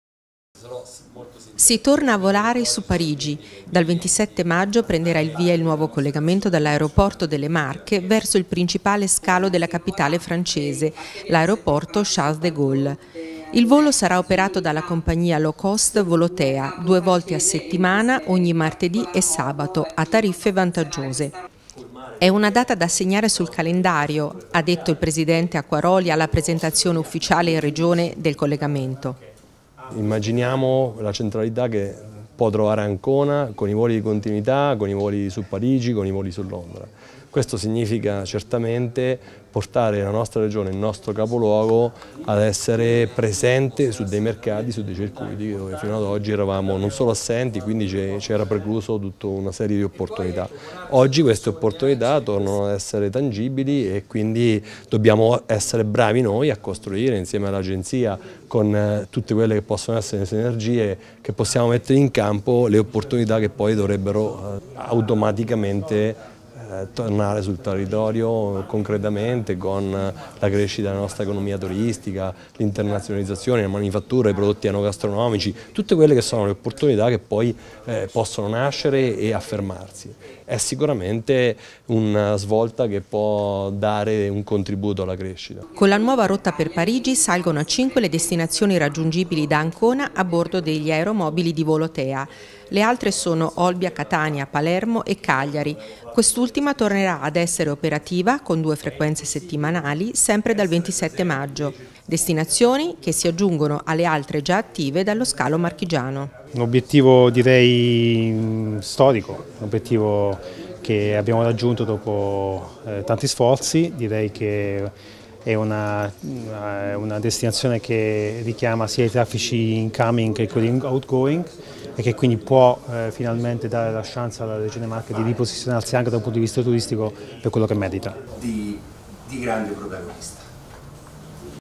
Si torna a volare su Parigi
Acquaroli: “Una grande opportunità per le Marche. L’aeroporto è uno snodo fondamentale per ridare centralità al nostro territorio”